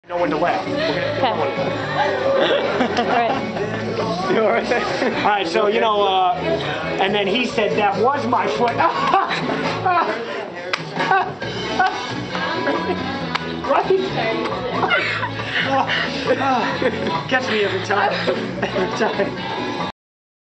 Richs Jokes... oh gosh. :P
lmao, soo everytime Nicole would go to take a picture with rich she would laugh so we came up with the idea to take a video oh laughing. :P